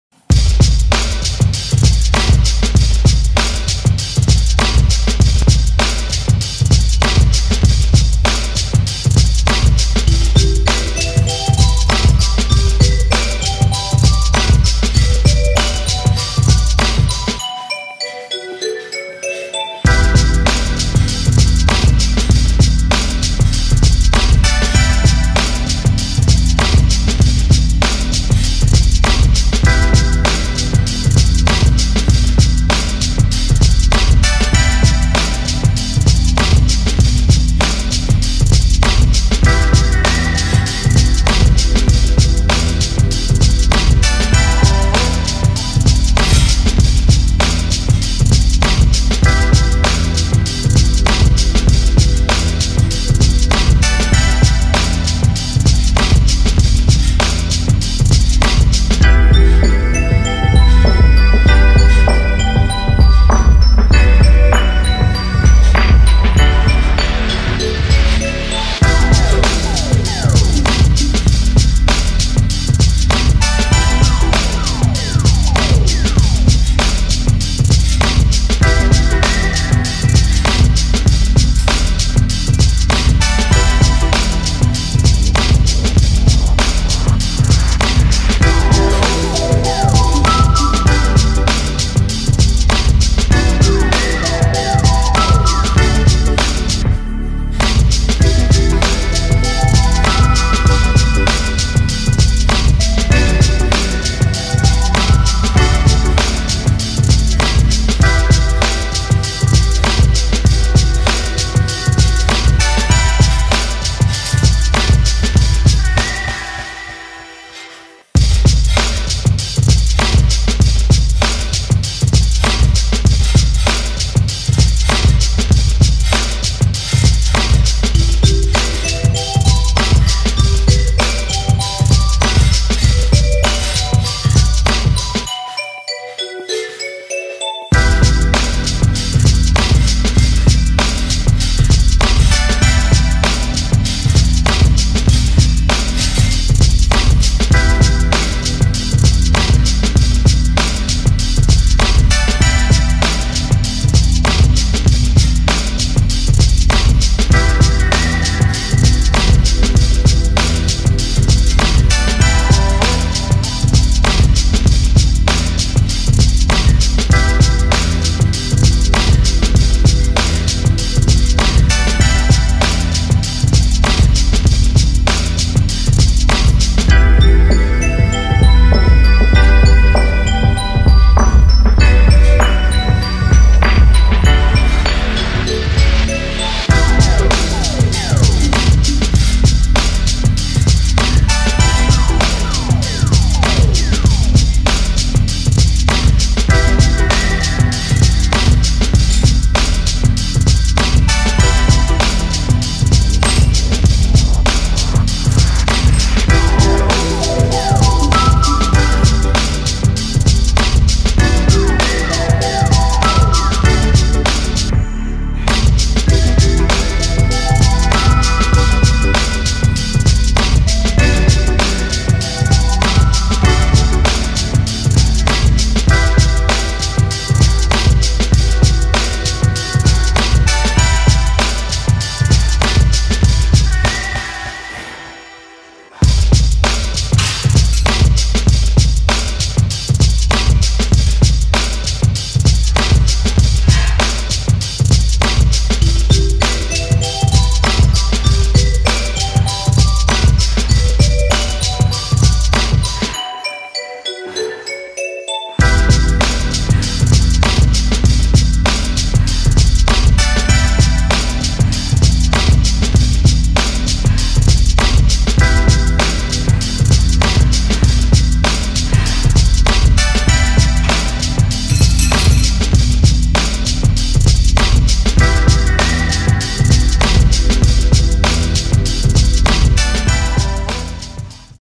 Different curl.
One arm cable curls, with burnout at end.